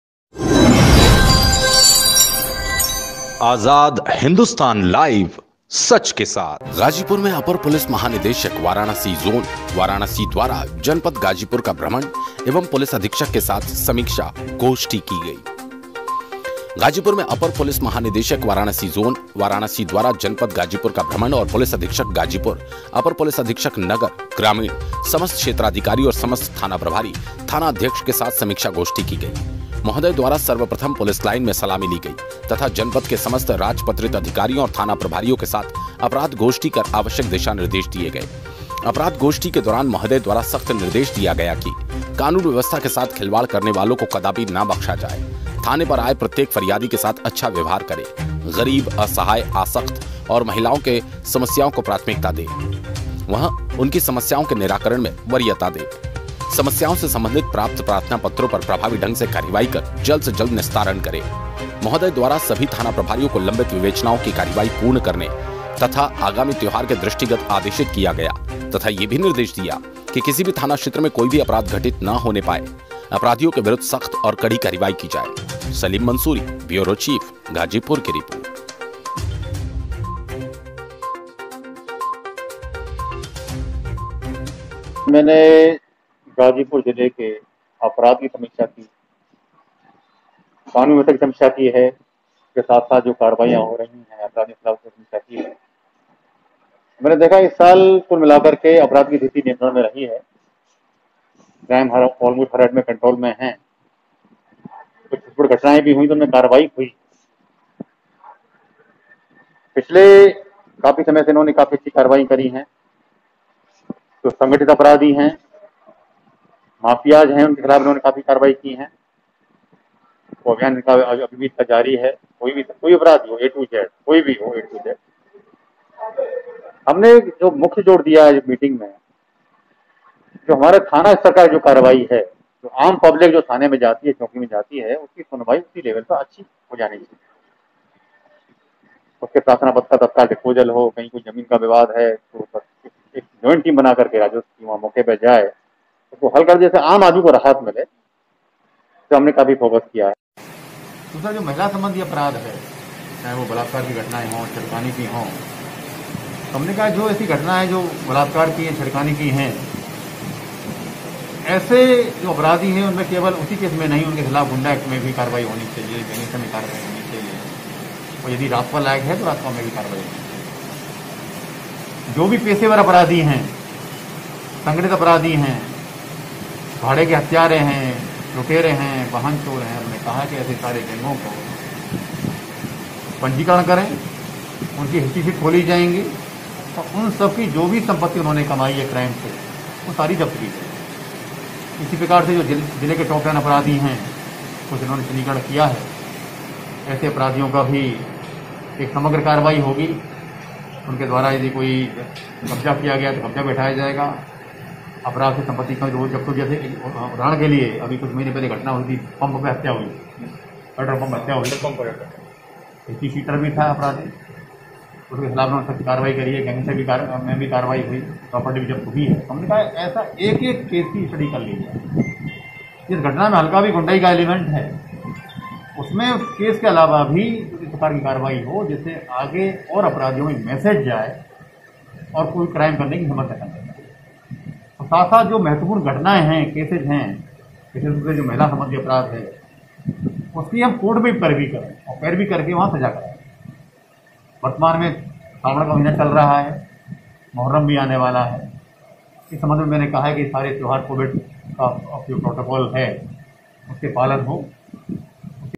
अपर पुलिस महानिदेशक वाराणसी जोन, वाराणसी द्वारा जनपद गाजीपुर का भ्रमण
गाजीपुर में अपर पुलिस महानिदेशक  वाराणसी जोन, वाराणसी  द्वारा जनपद गाजीपुर का भ्रमण एवं पुलिस अधीक्षक  के साथ समीक्षा गोष्ठी की।